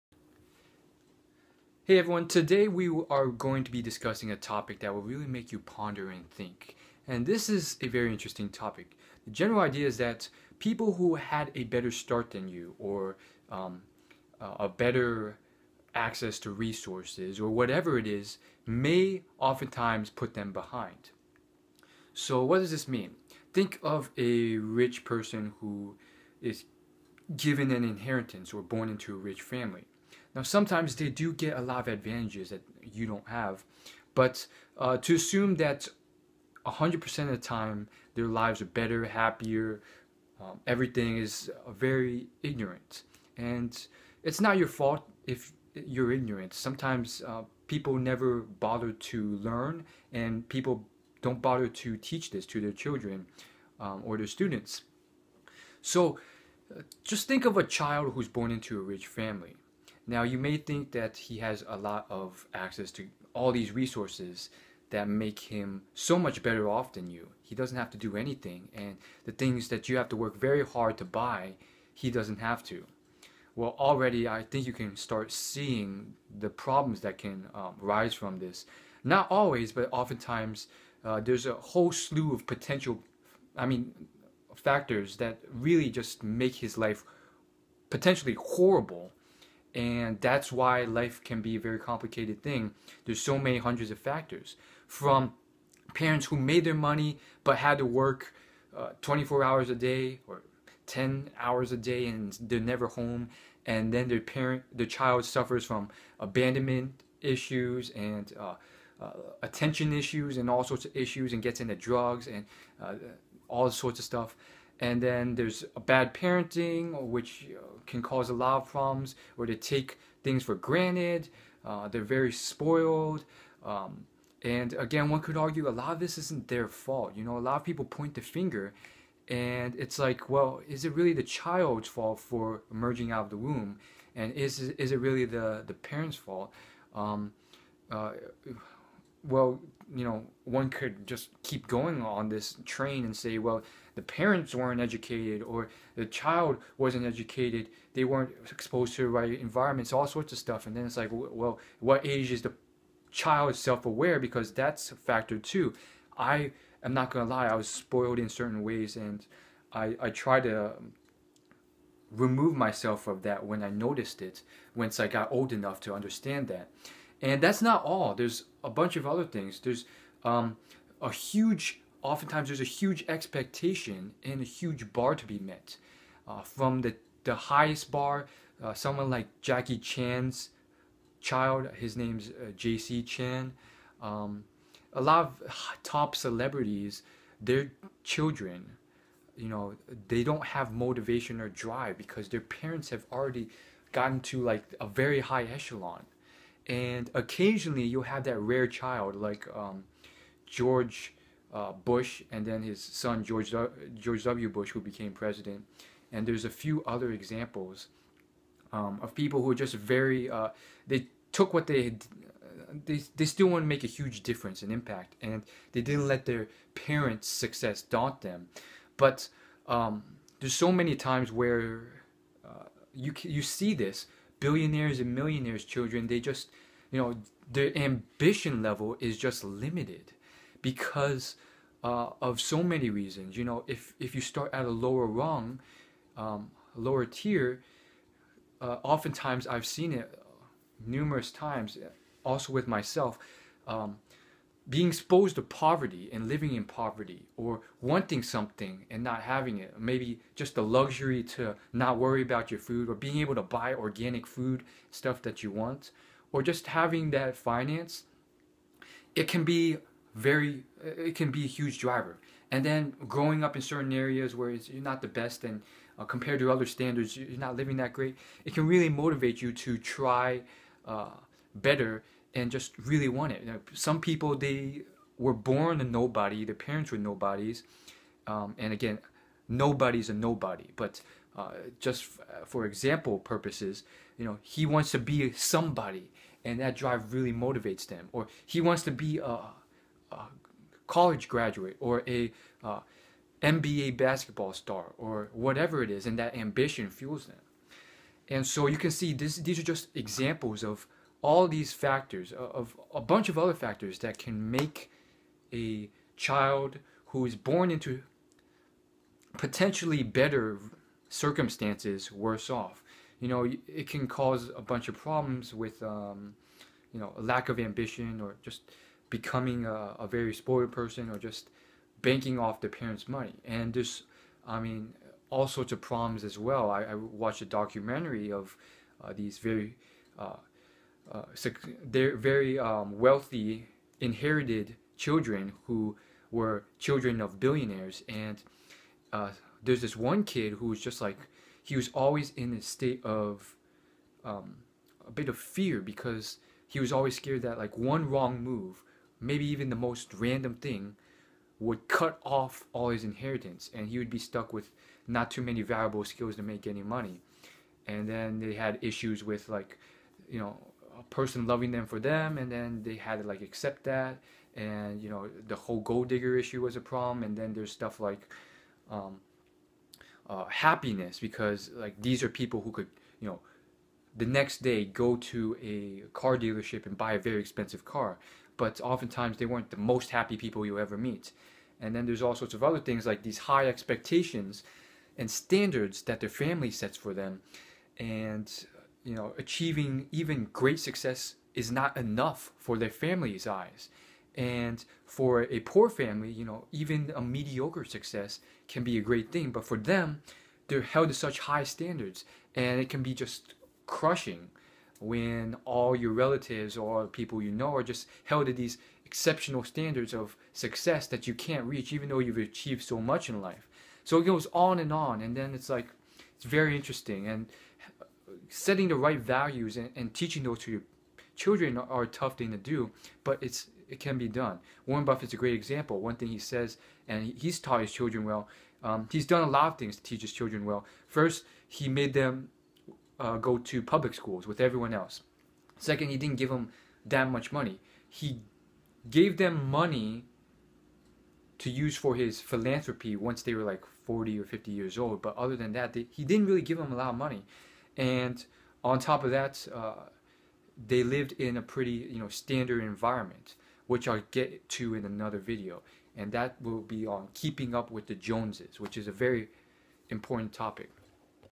How To Not Be Jealous Of Other People's Advantages In Life
Today, we talk about inherited traits and things people take for granted.